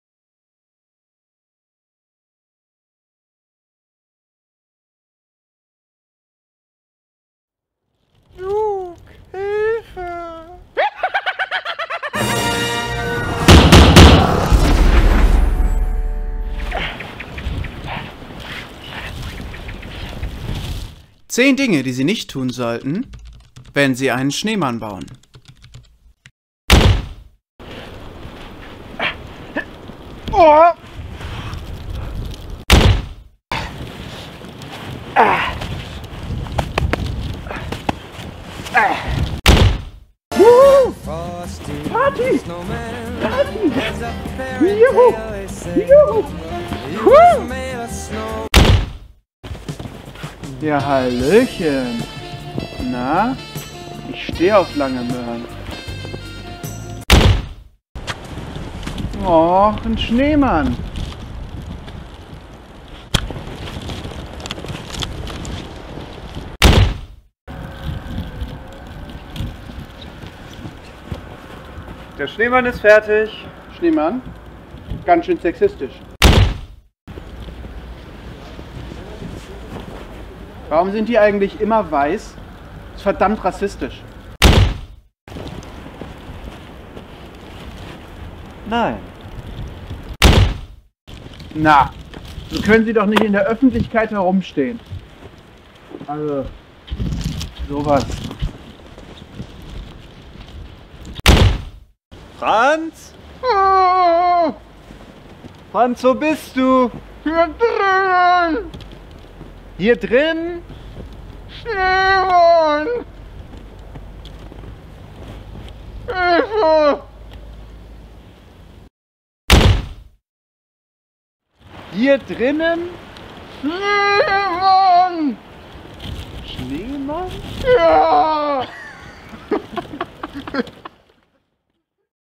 10 Dinge, die Sie nicht tun sollten, wenn Sie einen Schneemann bauen - FVR
Winter! Schnee! Spaß!
Einen Schneemann bauen – was kann da schon schiefgehen?

Wir haben eine kurze Liste an Beispielen zusammengestellt, die man beim Schneemann bauen oder begegnen besser unterlassen werden sollten.

Okaay, um ehrlich zu sein hat es seit 10 Jahren mal wieder geschneit – das musste ausgenutzt werden. Also ein Schnee-Spezial Video.

Wir bedanken uns bei Michael Kessler für die ursprüngliche Idee, mit der Bitte uns nicht zu verklagen.
Wir bedanken uns bei den Erstellern sämtlicher mehr oder weniger lizenzfreien Tönen und Liedern.
Danke an Krostiffer.

Eine CR●IB Presentations Zusammenarbeit
Ein FVR Productions Video
Copyright FVR-Cooporation